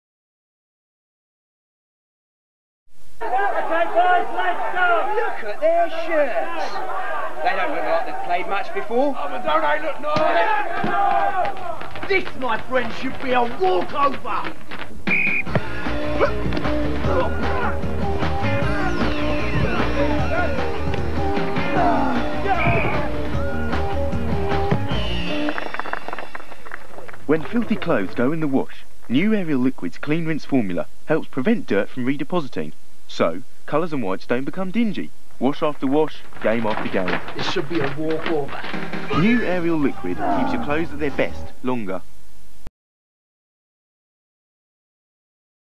1998: Ariel Futur Liquid [Rugby Game]
Title: Rugby Game
Brand: Ariel
Advertiser/Client: Procter & Gamble
Agency: -
Media: TV Advert
Directed By: -
Featuring: Toni Arthur
Guest Appearance: Meryl Payne
Voice Over: -
Year: 1998
Location: -
Country: UK
Washing Machine: -
Model: -
Slogan: Ariel Liquid Keeps Your Clothes At Their Best Longer.